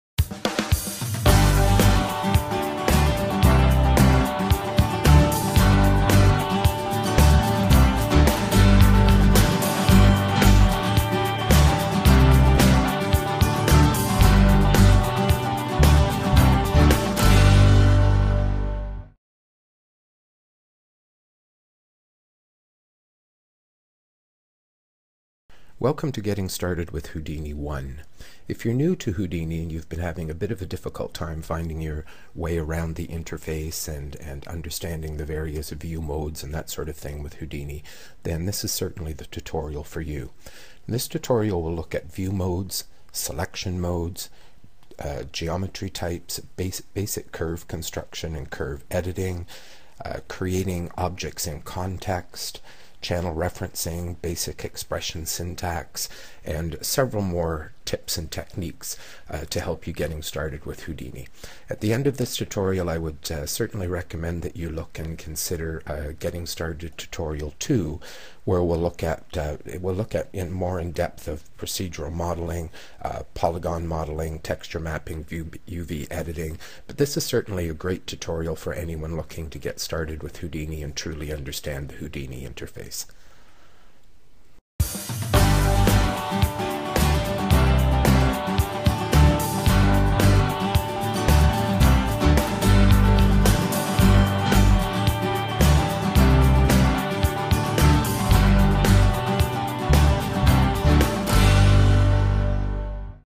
Introduction to Houdini 12
Ray Corbett returns with a fabulous set of 2 training videos for anyone needing a basic introduction to the interface and essential tools and concepts of this incredible software. Nearly 4 hours in total, this is a must-have if you are looking to take up Houdini.